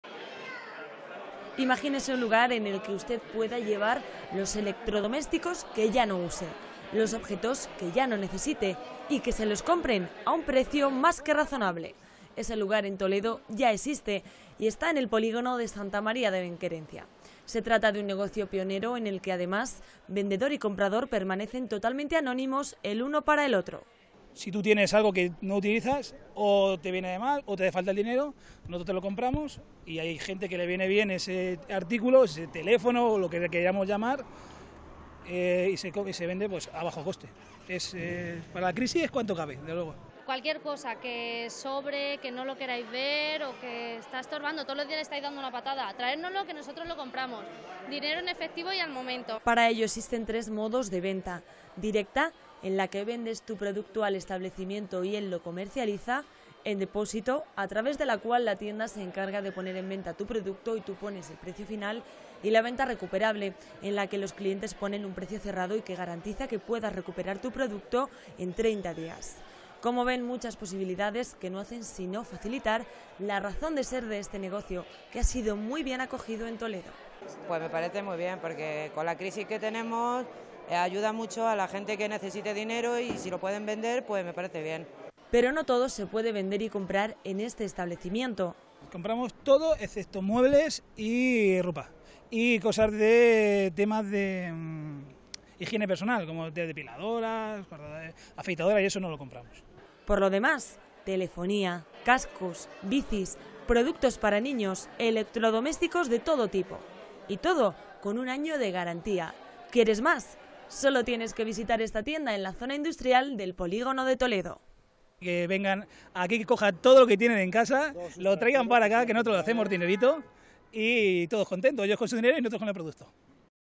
Un nuevo negocio que no va a pasar desapercibido en la ciudad de Toledo
Y cambiamos de asunto para acercarles la metodología de un nuevo negocio que no va a pasar desapercibido en la ciudad de Toledo. Se llama "Cash Money"  y en él puedes revalorizar los productos que ya no quieras en casa.

Imagínese un lugar en el que usted pueda llevar los electrodomésticos que ya no use, los objetos que ya no necesite, y que se los compren a un precio más que razonable. Ese lugar en Toledo ya existe, y está en el Polígono de Santa María de Benquerencia.

Se trata de un negocio pionero en el que, además, vendedor y comprador permanecen totalmente anónimos el uno para el otro.

Para ello existen tres modos de venta. Directa, en la que vendes tu producto al establecimiento y él lo comercializa. En depósito, a través de la cual la tienda se encarga de poner en venta tu producto y tú pones el precio final. Y la venta recuperable, en la que los clientes ponen un precio cerrado y que garantiza que puedas recuperar tu producto en 30 días.

Como ven, muchas posibilidad que no hacen si no facilitar la razón de ser de este negocio, que ha sido muy bien acogido en Toledo.

Pero no todo se puede vender y comprar en este establecimiento.

Por lo demás, telefonía, televisores, cascos, bicis, productos para niños, electrodomésticos de todo tipo. Y todo con un año de garantía. ¿Quieres más? sólo tienes que visitar esta tienda en la zona industrial del polígono de Toledo.